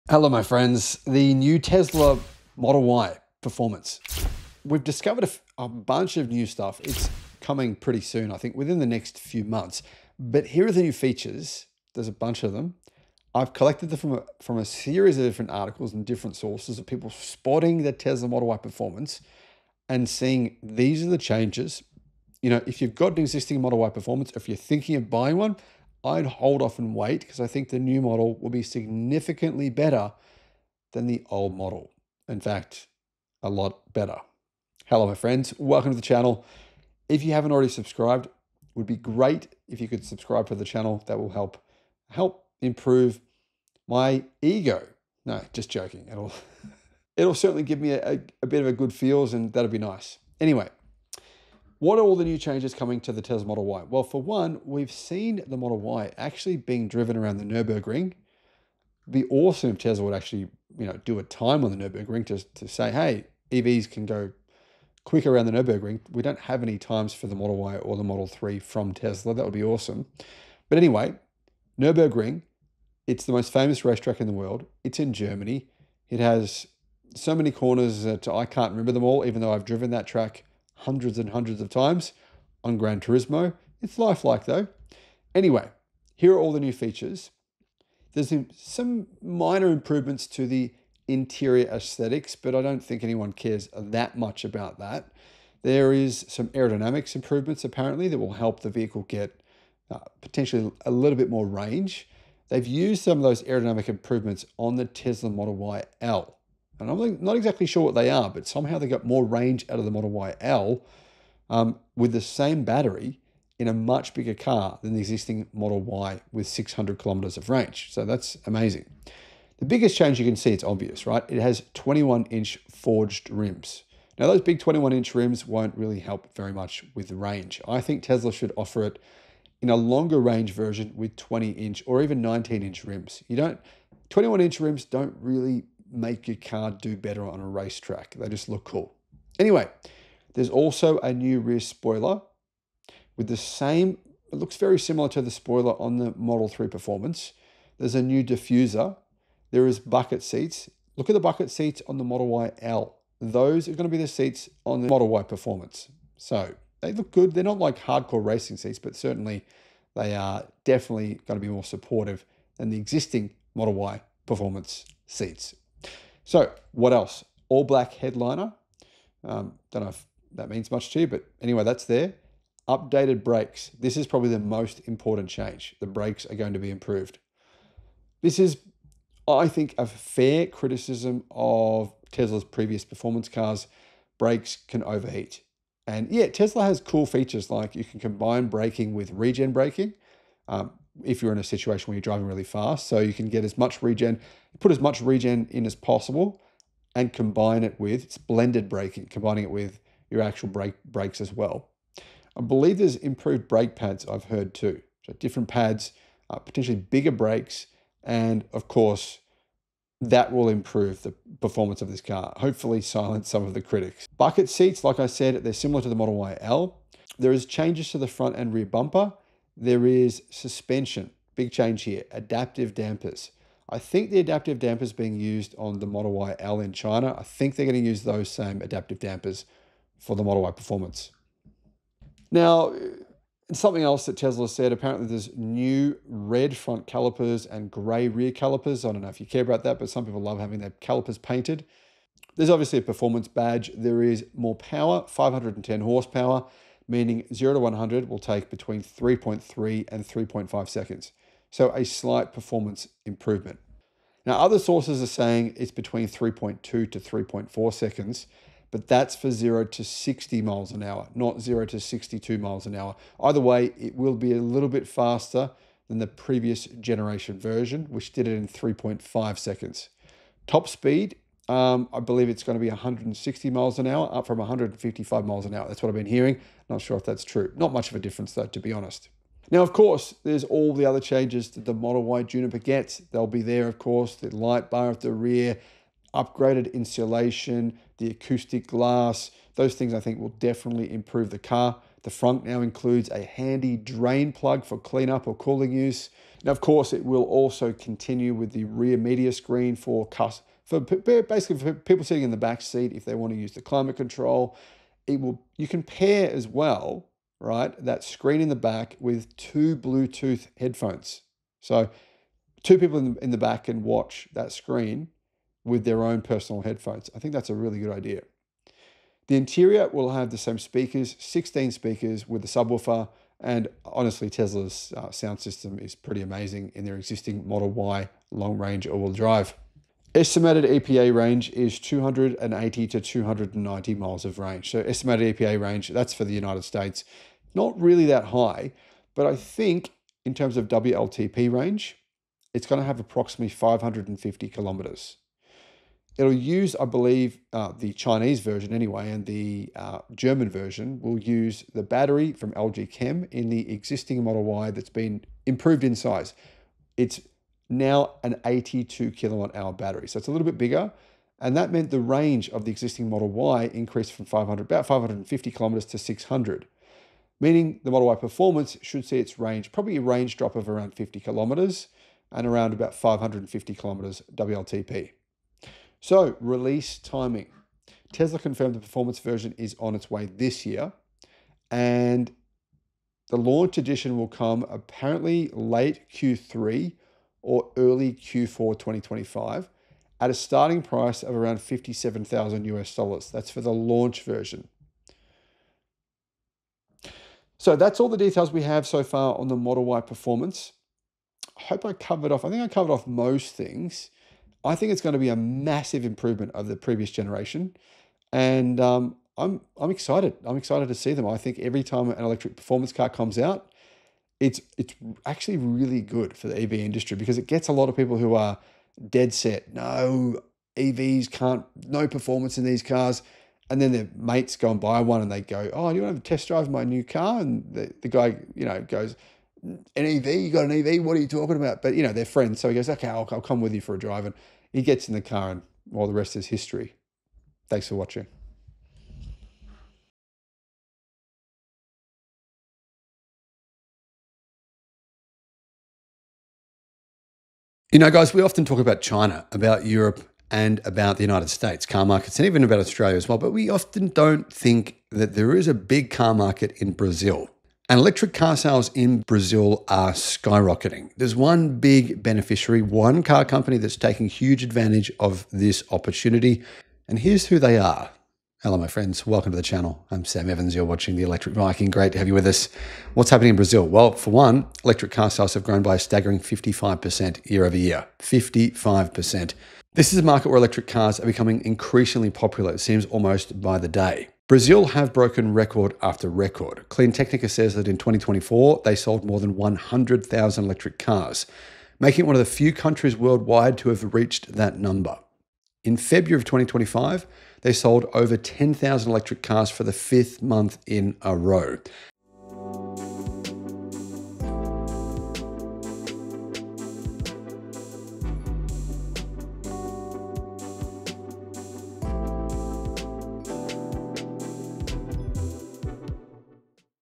Why the Next Tesla Model Y Performance Will Be a Game-Changer
The upcoming Tesla Model Y Performance is set to redefine the compact SUV segment with rumored faster acceleration, improved efficiency, and upgraded battery tech. If Tesla delivers, it could outclass rivals like the BMW iX3, Xpeng G7, and even Xiaomi’s YU7—cementing the Model Y as the ultimate EV family performance SUV.